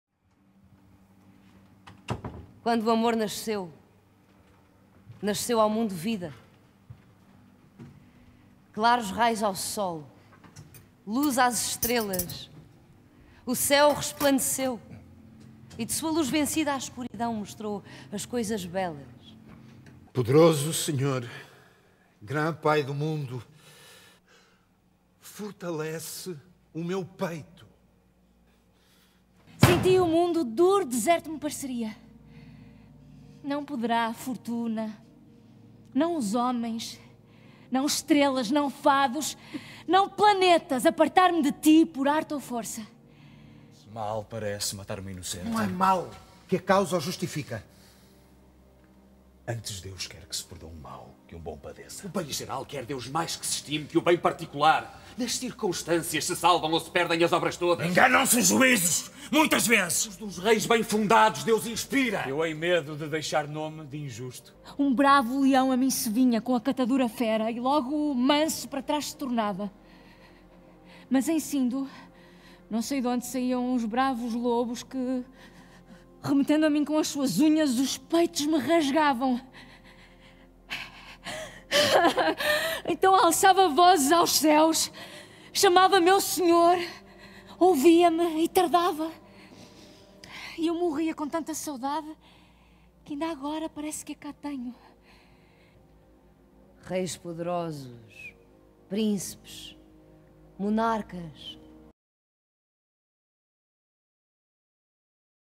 🎭Castro. Cativo é, quem de si se vence 🎭
Con Castro (1598), del poeta António Ferreira, Nuno Cardoso se instala por primera vez en el territorio de un canon de dramaturgia portuguesa, pionero de la tragedia clásica en Portugal. Y quiere habitar esta ficción literaria, ofreciéndole una lectura particular del drama histórico/leyenda/mito de los amores de Pedro y de Inés, para darle «ver con otros ojos», revelando a ella la modernidad y densidad intrínseca, velada por la poesía del lenguaje y por la elocución. Un inmenso escenario-casa-país, una especie de maqueta gigante de los espacios de acción, célula familiar primordial y claustrofóbica, nos pone en la cara de la intimidad concreta de los personajes que se revelan cautivos y su irreductibilidad.

En Castro, como en La muerte de Danton, la cuestión de la utopía (del amor, como de la revolución) es crucial. Es su negro de adentro hacia fuera lo que está expuesto: el amor / deseo y el poder como vicio y el caos, como prerrogativa, impunidad y arrogancia, como ceguera que «oscurece de esa luz antigua el rayo claro». Y mientras este oscurecimiento nublaba la decisión y se replicaba, tiñendo el tejido familiar con sangre y venganza, en un peculiar desplazamiento del centro de Castro de Inés, y la razón del estado como ficción y moral, para Pedro, en su relación especular con su padre, Alfonso IV. «¿Qué estrella era tan oscura?».

En el marco de la 44 edición del Festival Internacional de Teatro Clásico de Almagro.